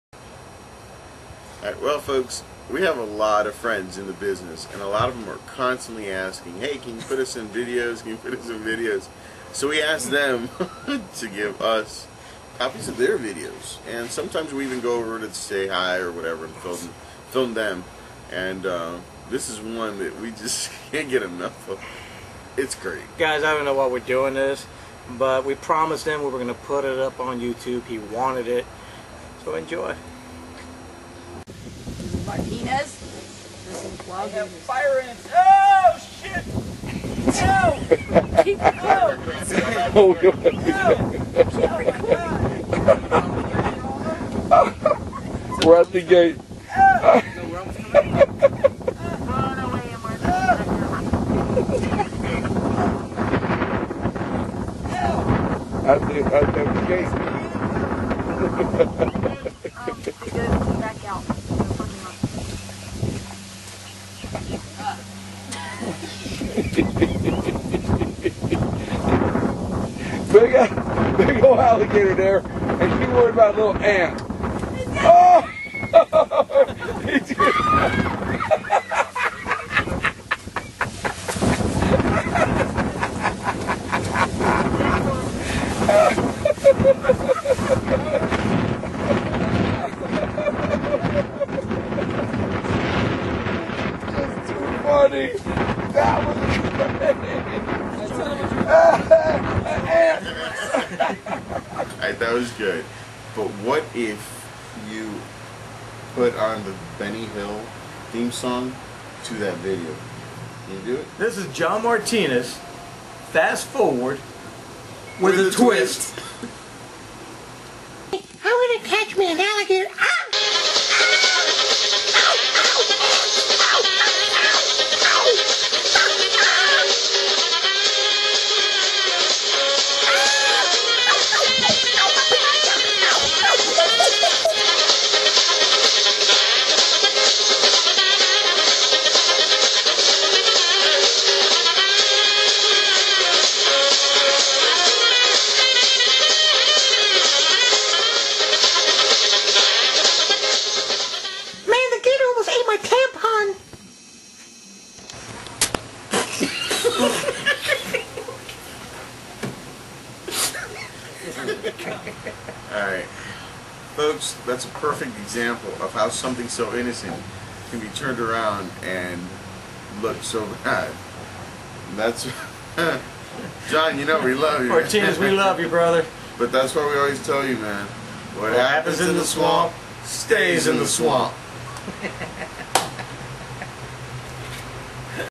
GUS ONEBEAR / BENNY HILL IN THE SWAMP..
GOOD FRIEND JOHN MARTINEZ GETS HIS WISH ON   YOUTUBE WITH A LIL SWAMP TWIST. THANKS TO GUS ONEBEAR AND JONATHAN CATTAIL..VERY FUNNYSTUFF GOTTA WATCH..WHAT HAPPENS IN THE SWAMP, STAYS IN THE SWAMP..